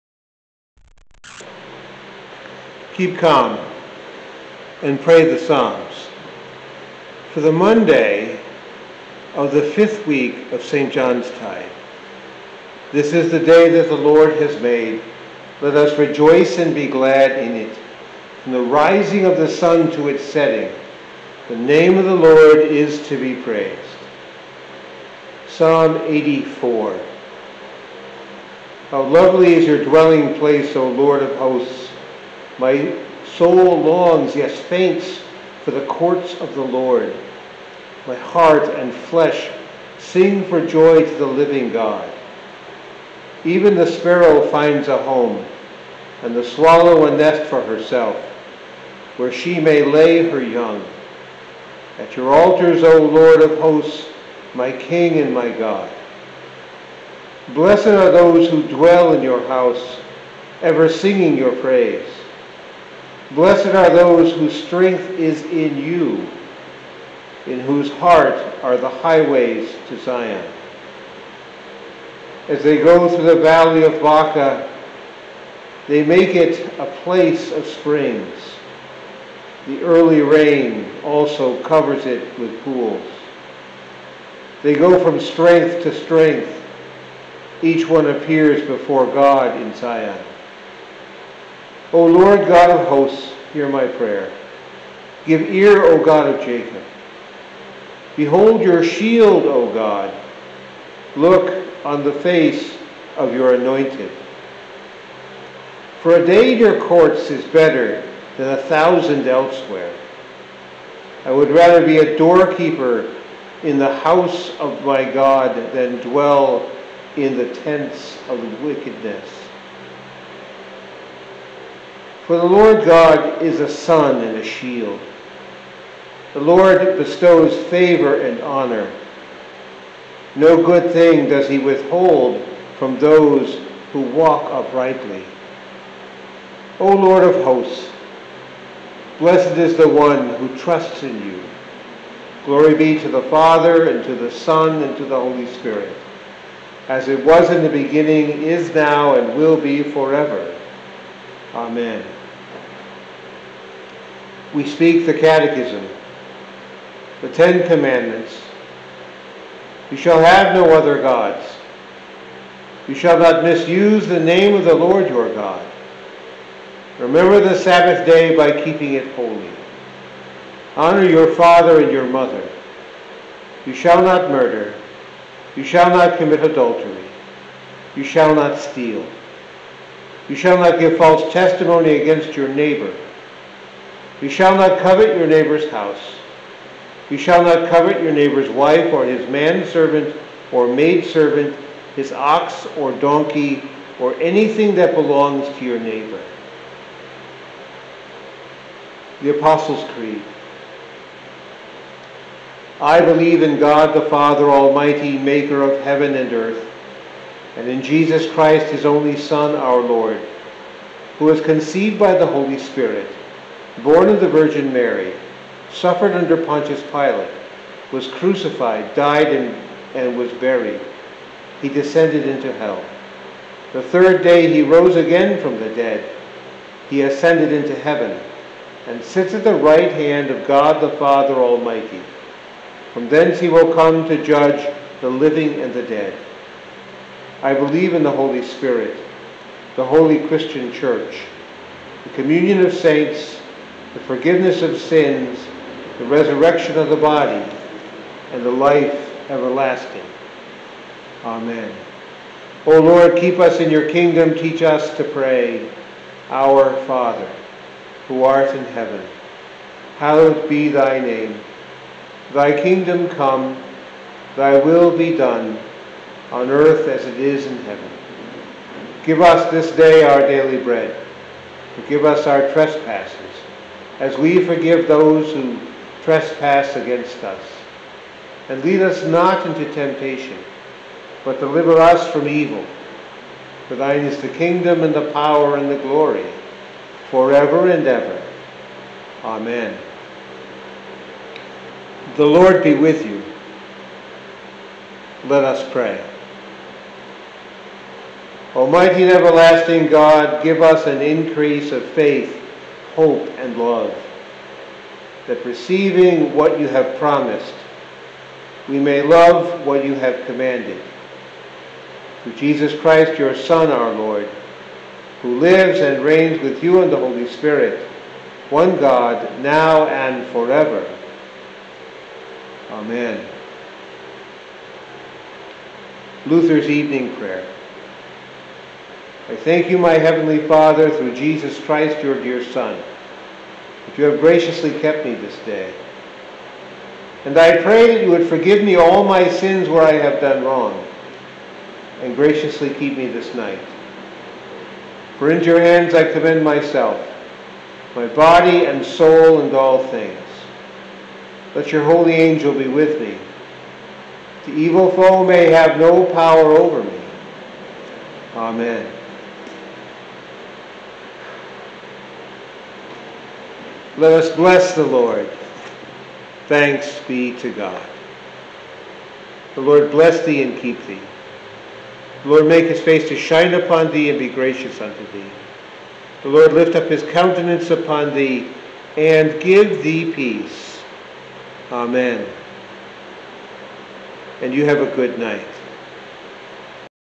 Keep Calm and Pray the Psalms Monday of the 5th week of St. John's Tide
The recording is from a new camcorder. the white noise is from  a fan running in the background. It was needed on a day when the heat index hit 100 in a church building that has no air conditioning.

Psalm 84
To the choirmaster: according to The Gittith. A Psalm of the Sons of Korah.
1 How lovely is your dwelling place,
        O Lord of hosts!
2 My soul longs, yes, faints
    for the courts of the Lord;
my heart and flesh sing for joy
    to the living God.

3 Even the sparrow finds a home,
    and the swallow a nest for herself,
    where she may lay her young,
at your altars, O Lord of hosts,
    my King and my God.
4 Blessed are those who dwell in your house,
    ever singing your praise! Selah

5 Blessed are those whose strength is in you,
    in whose heart are the highways to Zion.
6 As they go through the Valley of Baca
    they make it a place of springs;
    the early rain also covers it with pools.
7 They go from strength to strength;
    each one appears before God in Zion.

8 O Lord God of hosts, hear my prayer;
    give ear, O God of Jacob!                                      Selah
9 Behold our shield, O God;
    look on the face of your anointed!

10 For a day in your courts is better
    than a thousand elsewhere.
I would rather be a doorkeeper in the house of my God
    than dwell in the tents of wickedness.
11 For the Lord God is a sun and shield;
    the Lord bestows favor and honor.
No good thing does he withhold
    from those who walk uprightly.
12 O Lord of hosts,
    blessed is the one who trusts in you!